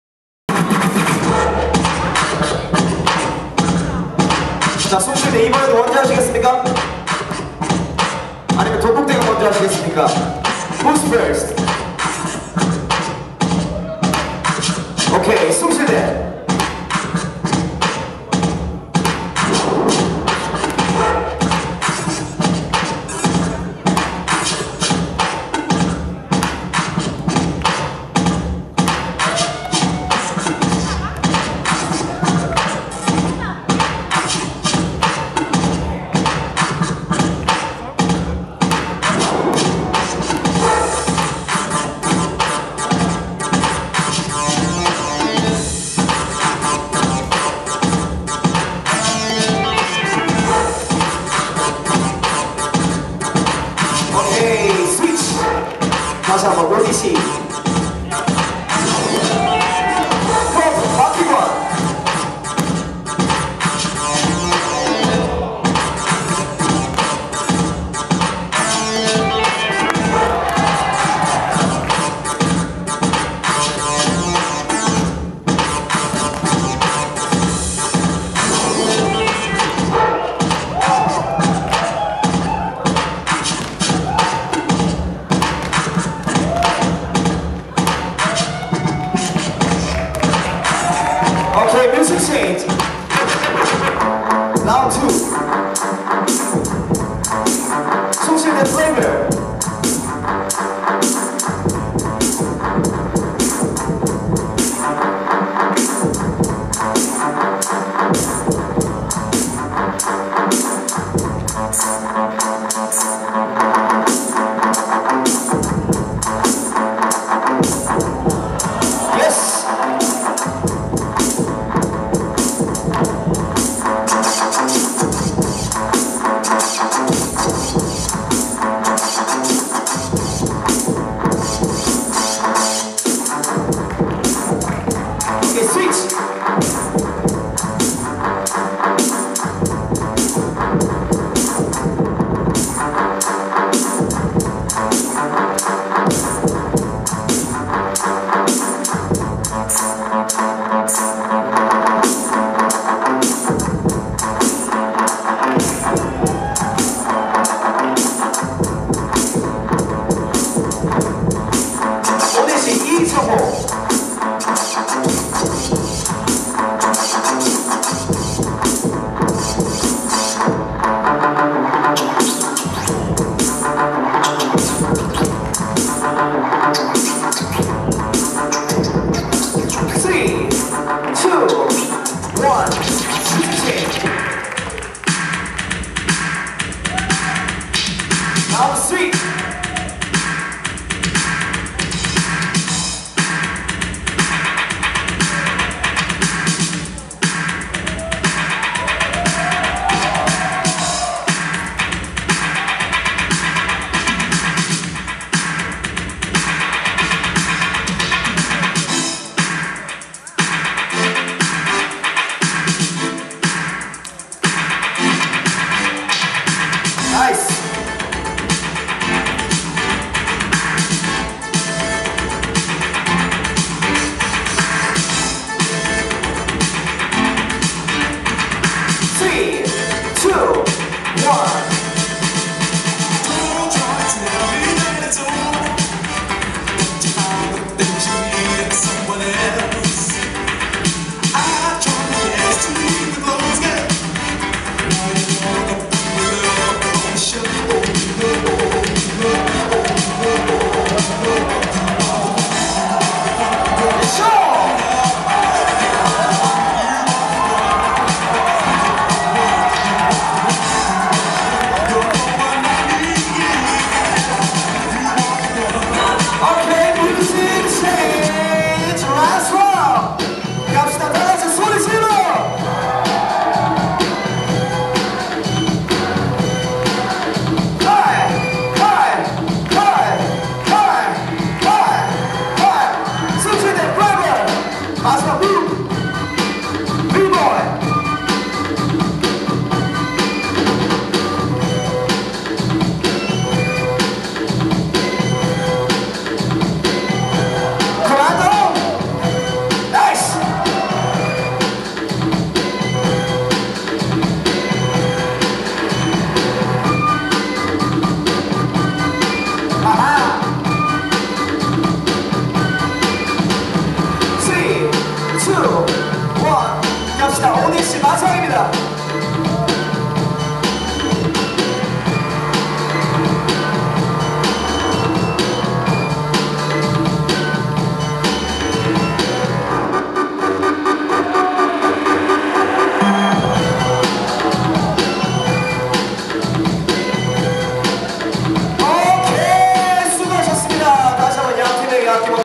Flavor vs ODC - Semi final @ are u ready track.06
UNIVERSITY DANCE CLUB PRIDE BATTLE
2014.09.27(SAT) 동국대학교 중강당

MC - DU LOCK(Funky street prince)
DJ - JAYGEE(Real marvelous)

JUDGEs
CRAZY KYO(B.W.B / Puzzler)
BEAST(TG Breakers / Leadmos crew)
HALO(Cube sound)

GUEST SHOW
TRIPLE B

BATTLE GUEST&OPENING SHOW
F.L.EX.(TRACK.05 WINNER)